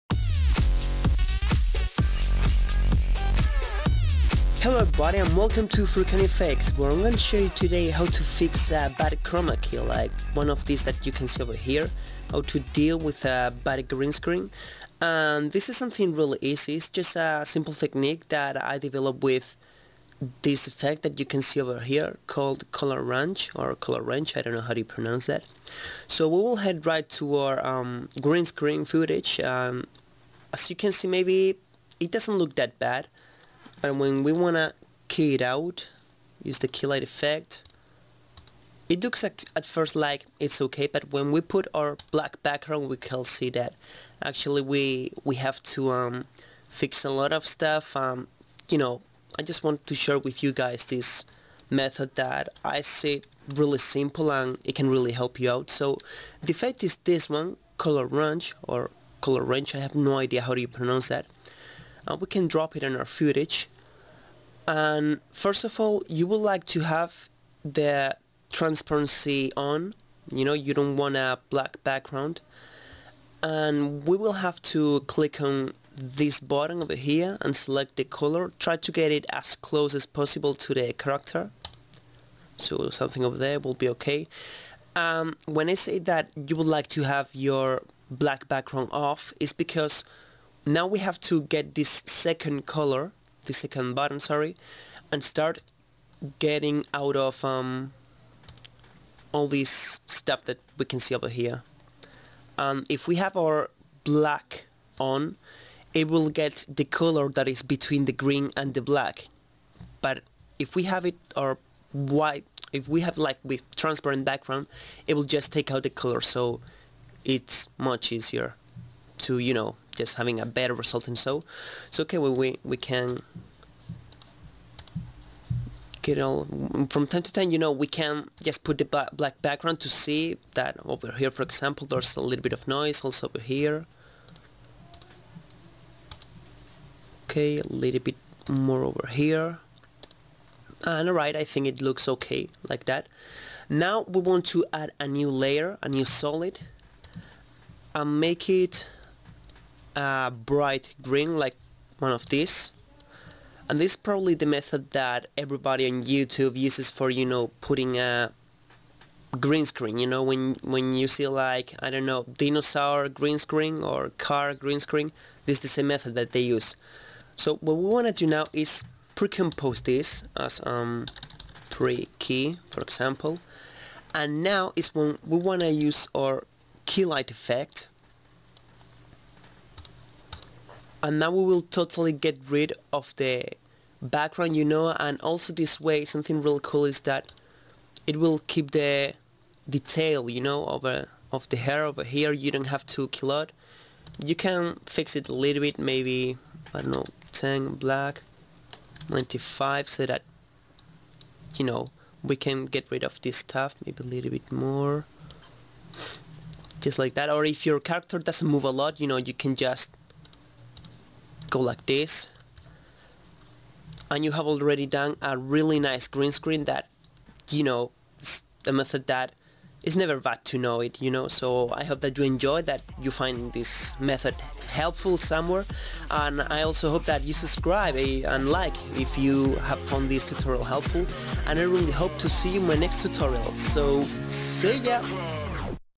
How to deal with a bad Chroma Key / Fix bad Green Screen Tutorial [After Effects]
I'm sorry for the horrible quality in this video, Camtasia Studio has updated and they've totally screwed up the software. Basically now you're not able to import any audio that is not .mp3, but when you import the .mp3 it sounds just horrible. I'll try to fix that for the next tutorial!

Follow me in the social media!